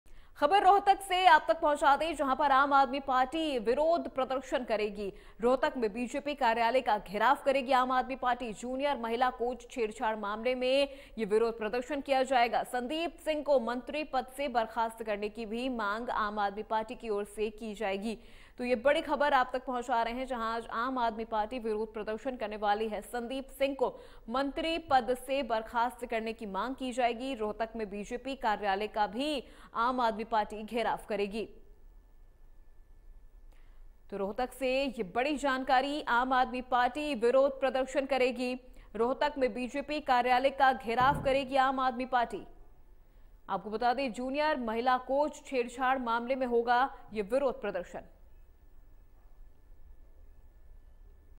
Haryana News : आम आदमी पार्टी करेगी विरोध-प्रदर्शन रोहतक में BJP कार्यालय का घेराव केरेगी ‘आप’
Haryana News : आम आदमी पार्टी करेगी विरोध-प्रदर्शन रोहतक में BJP कार्यालय का घेराव केरेगी ‘आप’| Hindi News